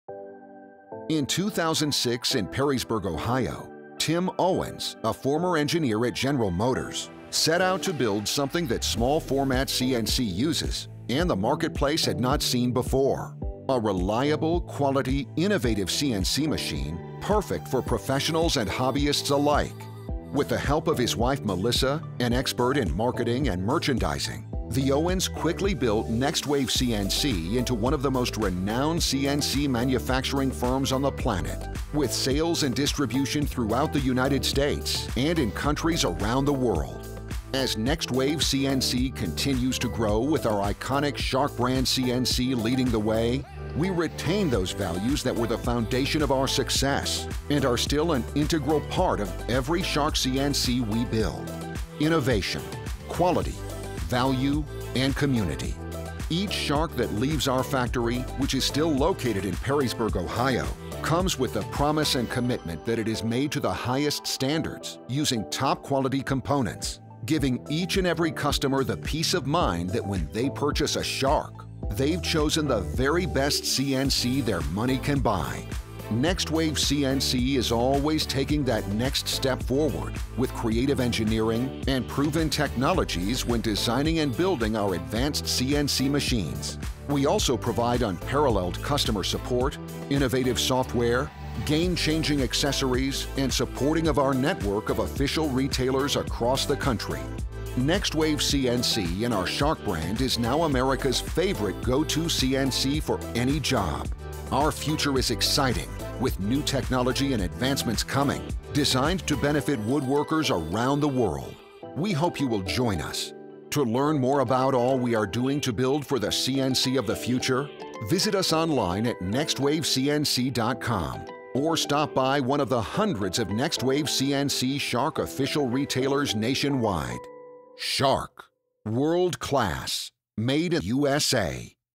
'Made in USA' is Not Just a Slogan
Outgrowing Your CNC - Why not SHARK SWAP it? Next Wave CNC will completely refurbish your SHARK SD100 and through our partnership with The Mill National Training Center in Colorado Springs, Colorado, donate it in your name. Shark Swap allows Next Wave CNC to continue to support high school technical education and shop programs focused on training and technology.